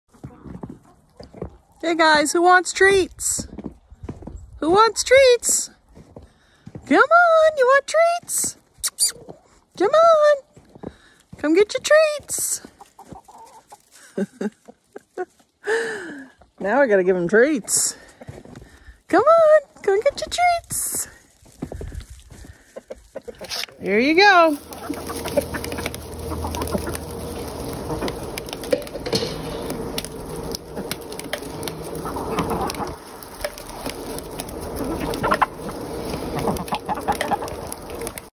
Spoiled Chickens Want Treats
A quick of the chickens following me for treats.